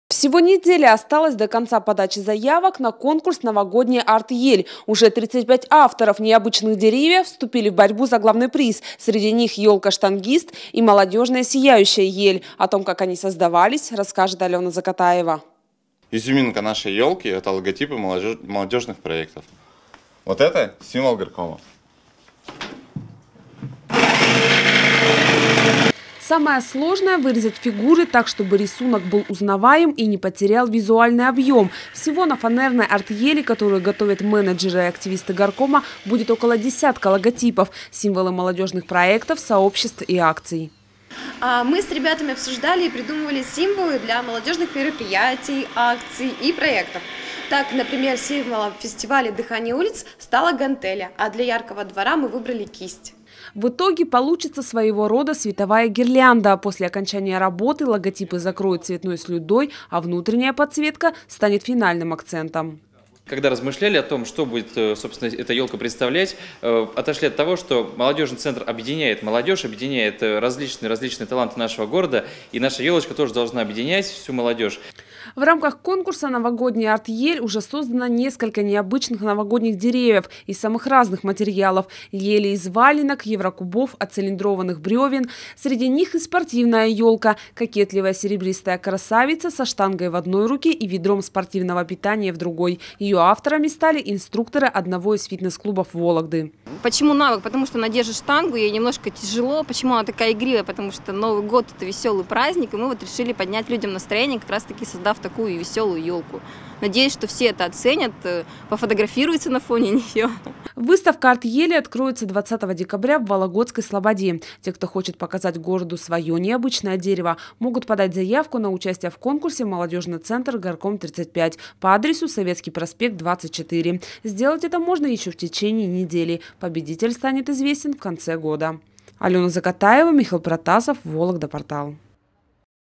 Всего неделя осталась до конца приема заявок на конкурс «Новогодняя АРТель»
Уже 35 авторов необычных деревьев вступили в борьбу за главный приз. В рамках конкурса создано несколько необычных новогодних деревьев из самых разных материалов. Это ели из валенок, еврокубов, оцилиндрованных бревен.